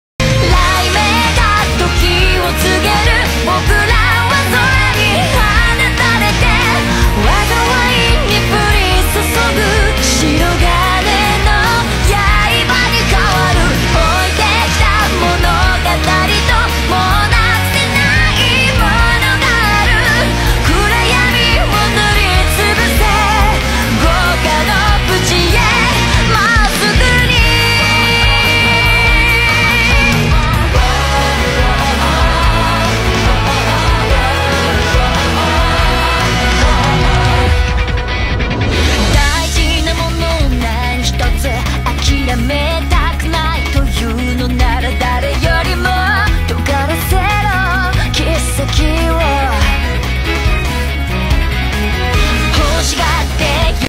Highlights making of leg warmers from Nezuko Cosplay. Full video wit
diy Highlights making of leg warmers from Nezuko Cosplay. Full video with detail and explanation as always on UTube :) .